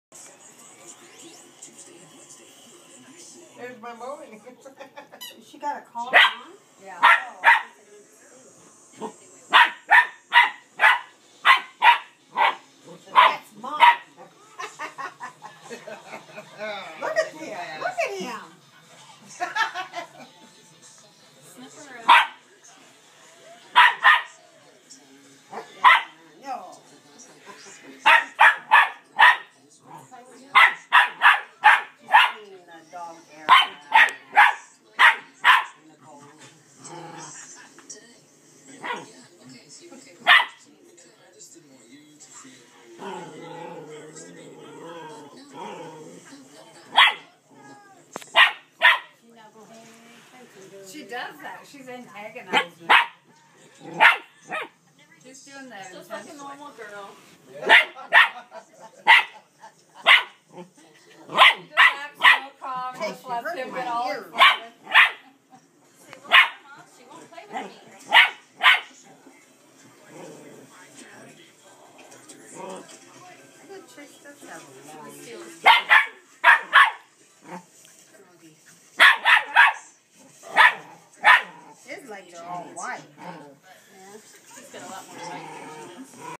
Chase barking at Bright Eyes
Chase is barking at his sister Bright Eyes trying to get her play. She was being stubborn. :)